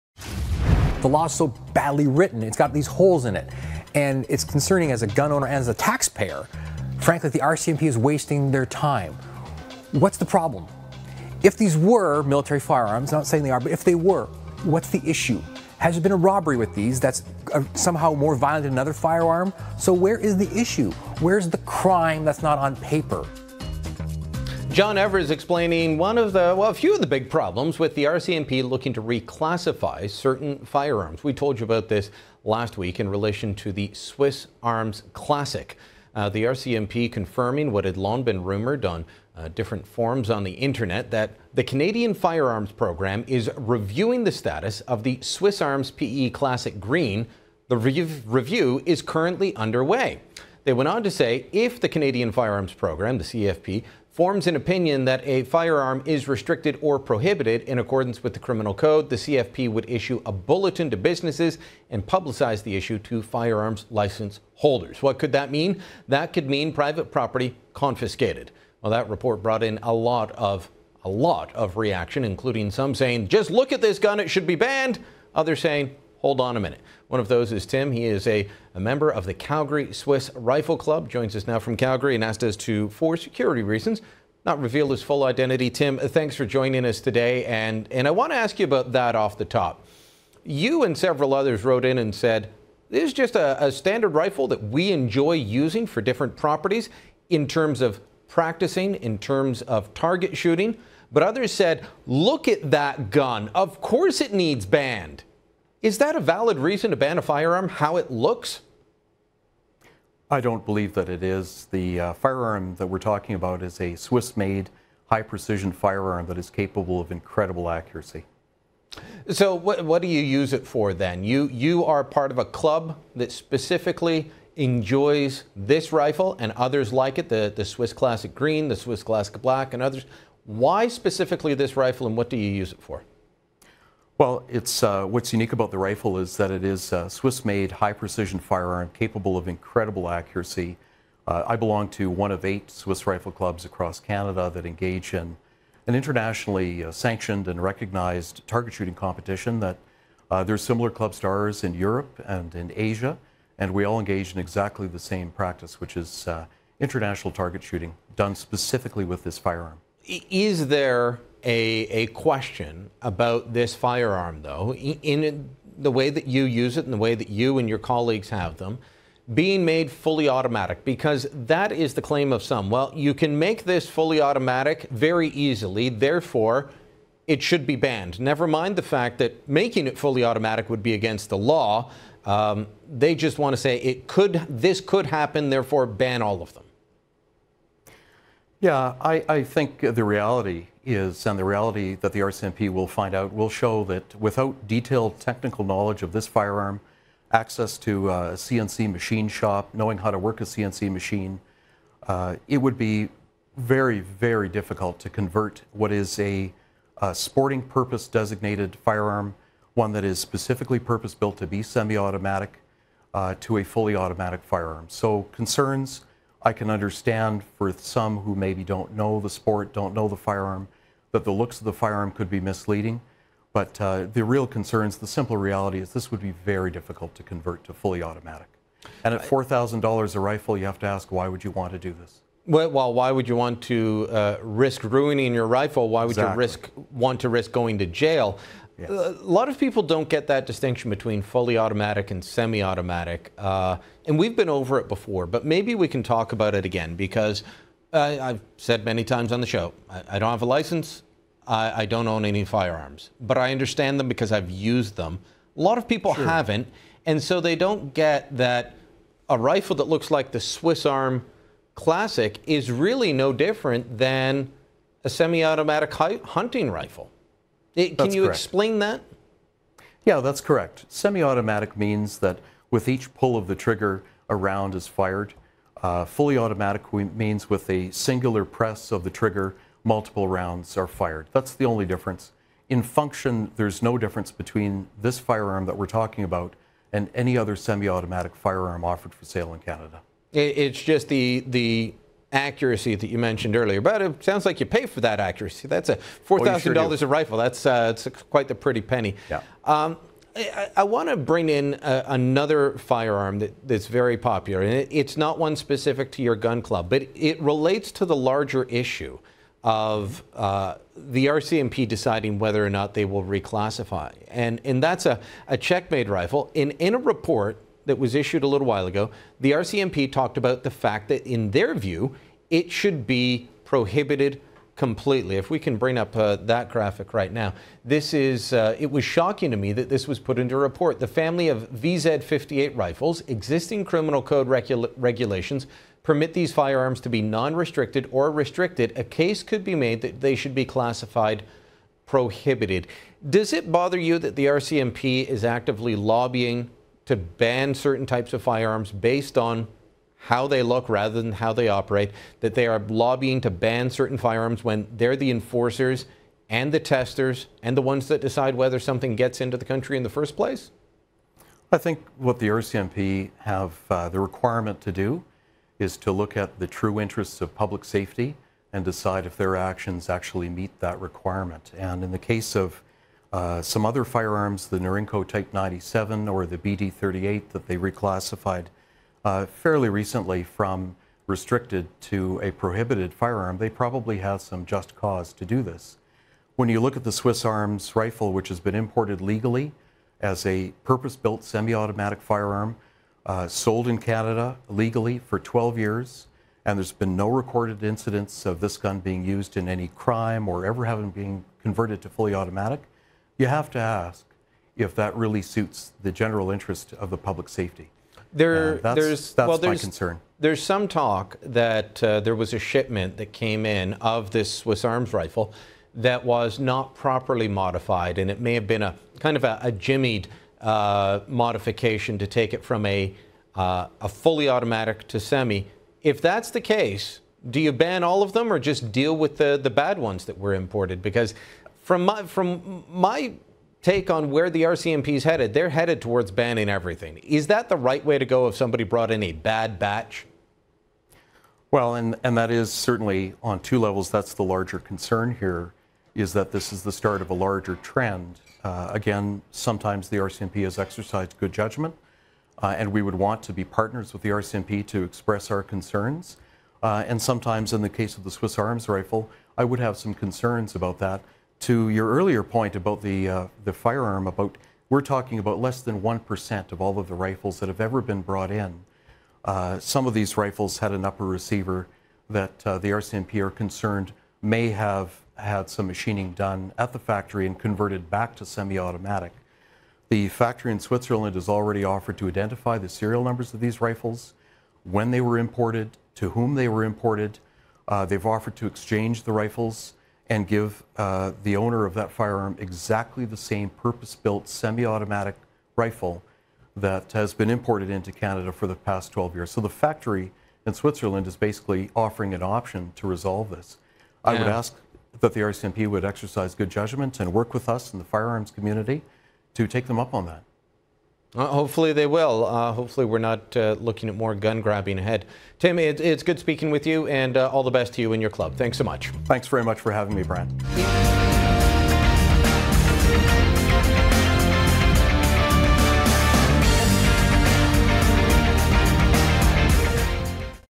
Rifle reclassification
A Calgary Swiss Rifle Club member shares his thoughts on the RCMP looking to reclassify certain firearms.